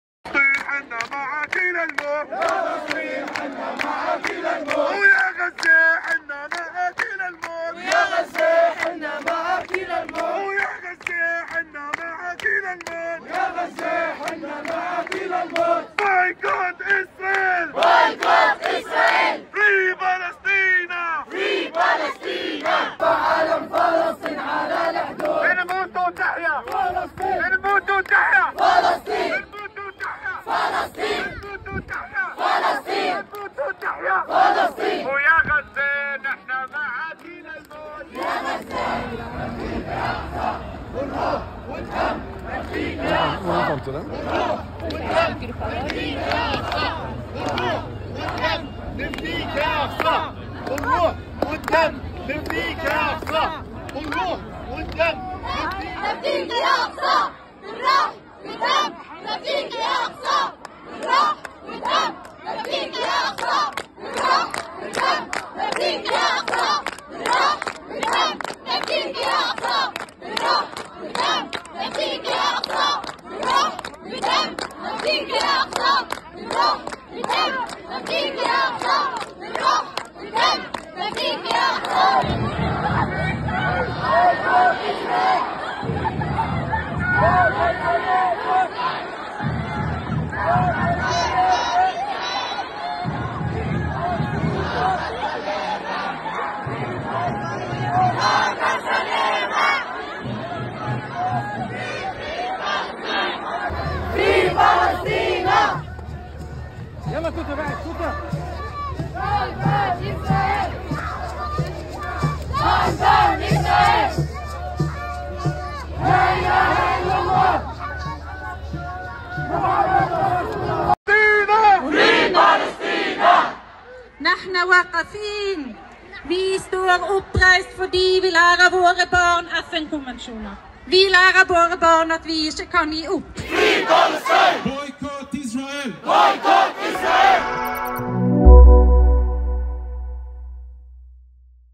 دعماً لفلسطين.. مئات العرب والنرويجيين يخرجون بمظاهرة حاشدة في مدينة أوسلو
دعماً لفلسطين.. مئات العرب والنرويجيين يخرجون بمظاهرة حاشدة في مدينة